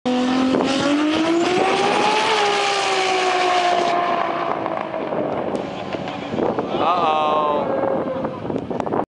Boost Logic final run TX2K12
mechanical failure sidelined the amazing Boost Logic Supra, already breaking their best time previously with a 7.7 @ 188mph